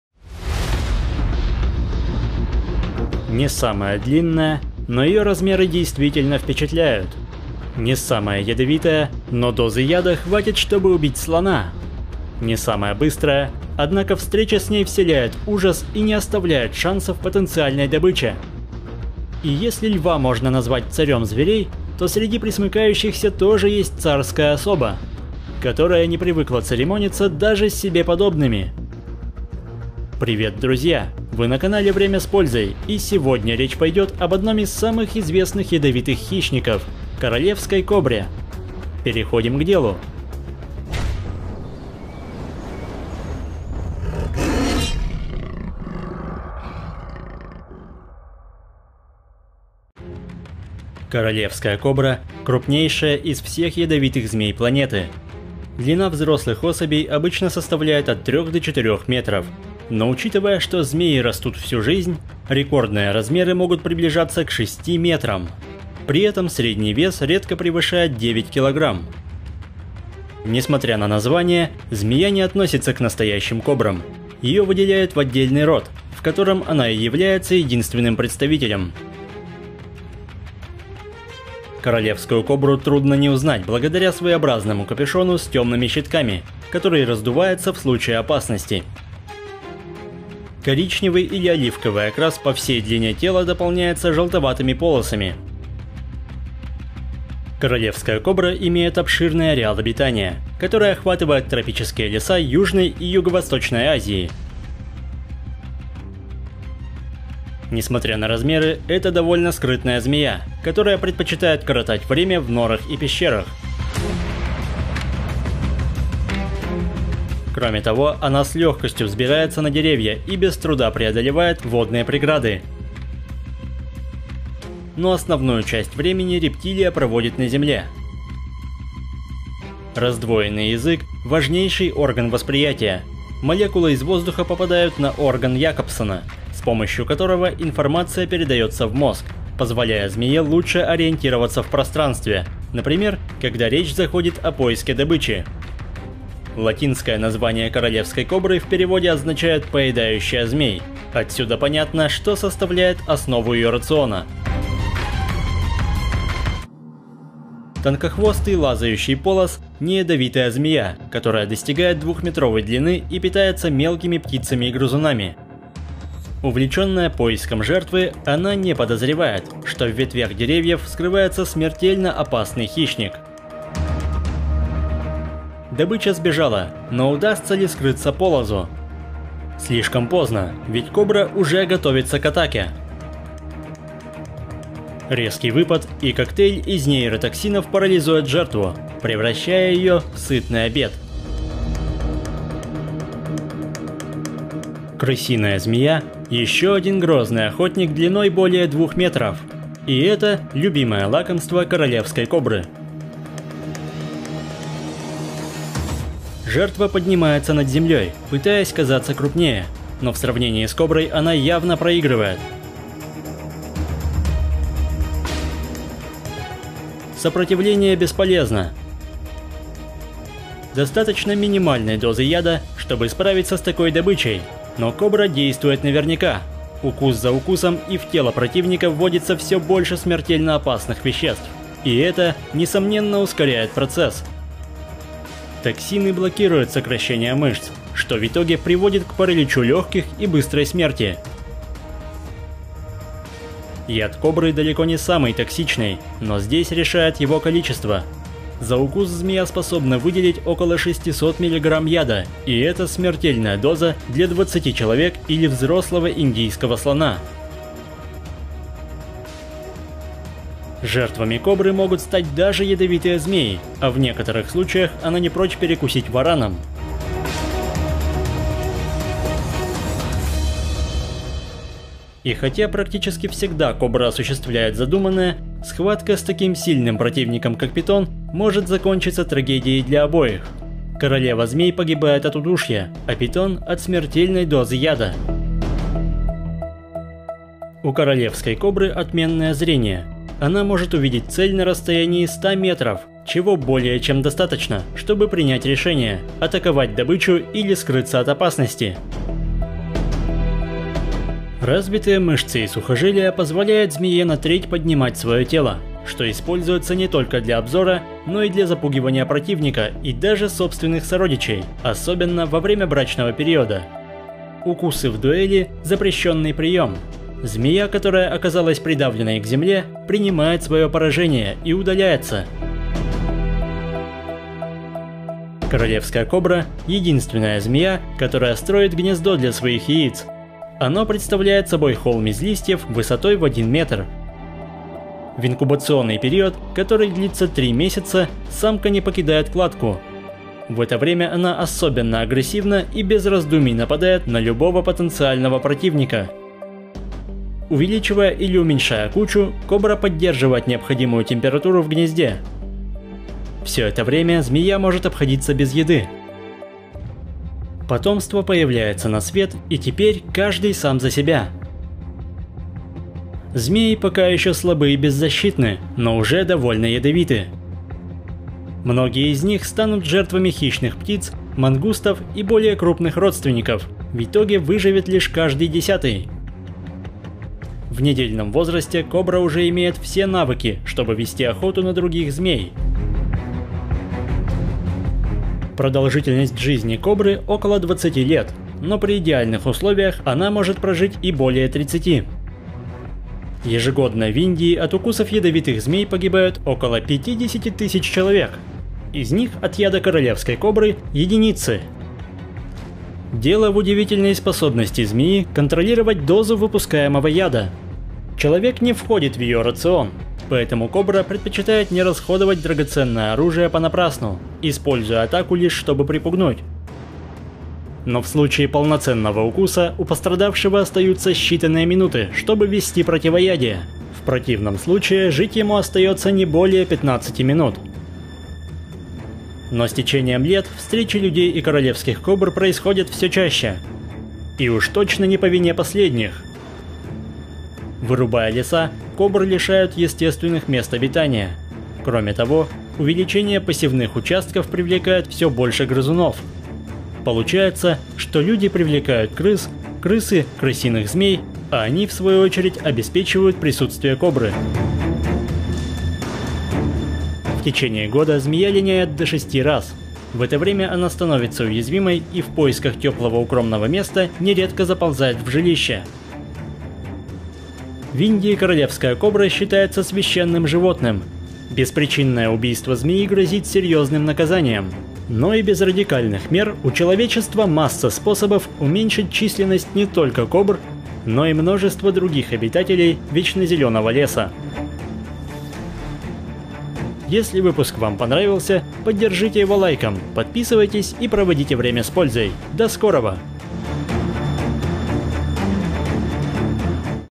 kingcobra King Cobra is the ultimate snake hunter! Long and poisonous mistress of the forest!
Perhaps the most famous poisonous snake, which is distinguished by its huge size, especially dangerous neurotoxins and an unusual diet, the main part of which is other snakes.  In addition, she can control the dose of poison, easily climb trees and build real nests!  Meet the king cobra.  Happy viewing